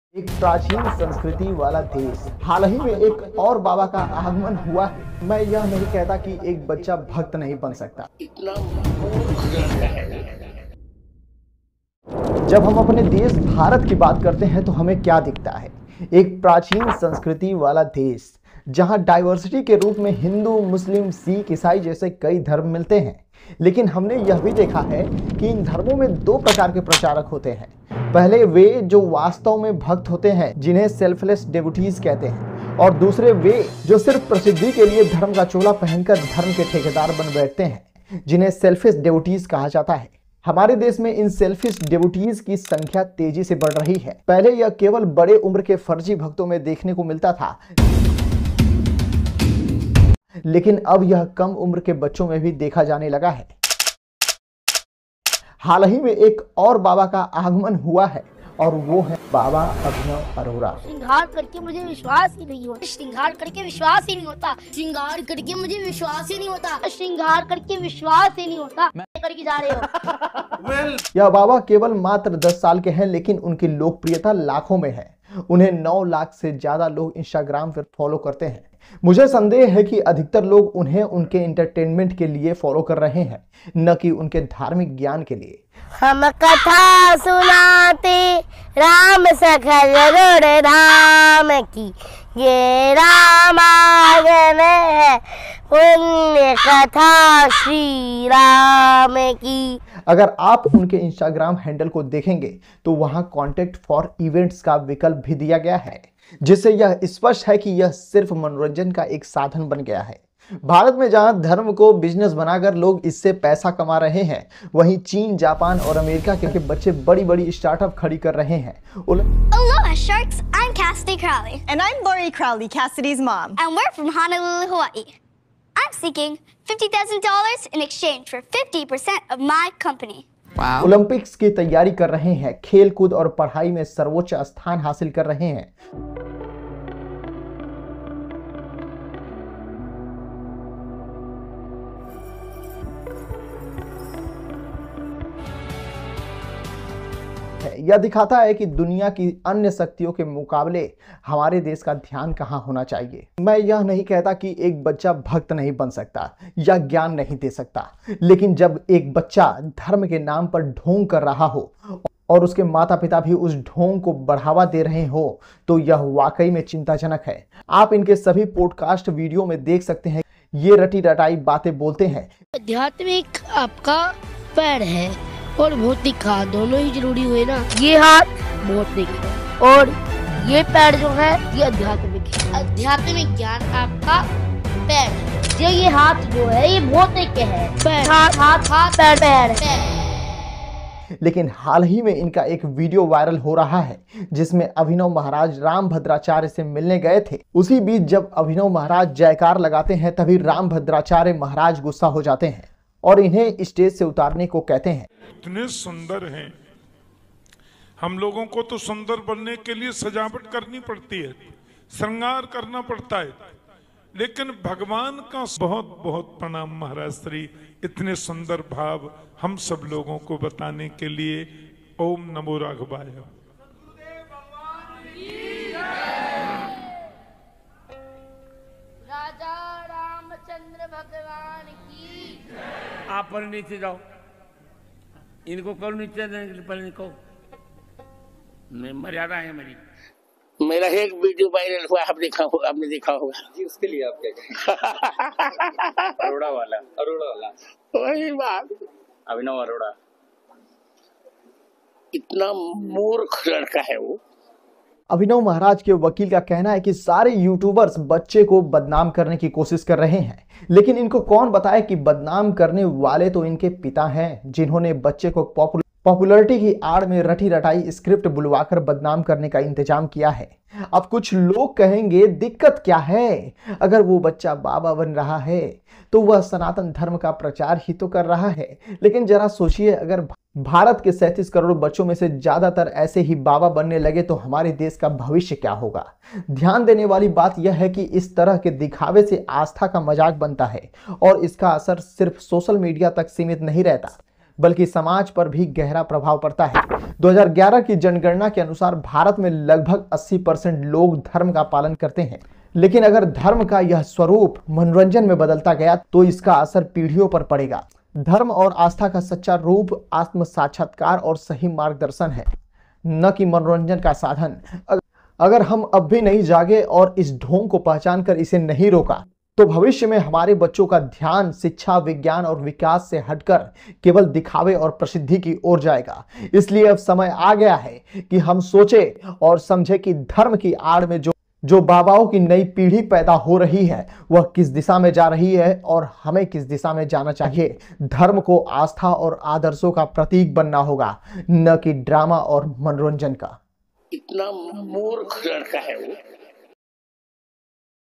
The End of Baba Abhinav Arora🚫| Rambhadracharya Vs Abhinav
Description:

India's ancient culture is a blend of diverse religions and beliefs, where true devotees have uplifted faith and spirituality. However, today, religion is increasingly becoming a means to gain fame and social media following. Recently, a young 'baba,' Abhinav Arora, just 10 years old, has gained over 9 lakh followers on social media. The question arises: are people following him for genuine spiritual knowledge, or simply for entertainment?

When children begin to play the role of "godmen," and parents encourage this, it raises serious concerns for our society's future. If we don't address this issue, our next generation may focus more on fame and appearances rather than education, science, and real progress. It's time we recognize and curb this facade. Faith should be a symbol of belief and ideals, not a source of drama and entertainment.